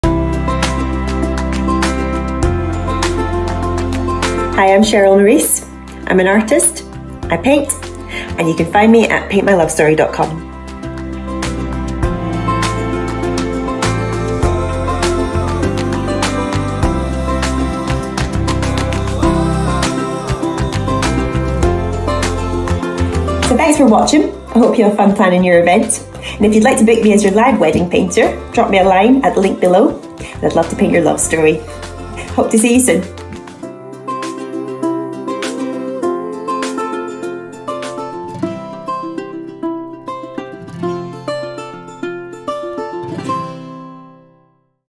Live Wedding Painter /Event Artist UK , Personal commissions something different for your wedding
Cheryl Morrice is an artist and Live Wedding Painter / Events Artist UK
Cheryl paints personal painting commissions

Taking bookings for UK Weddings for something different

Bespoke wedding packages available or wedding gift vouchers available to give as a wedding gift

Let Cheryl Paint your story !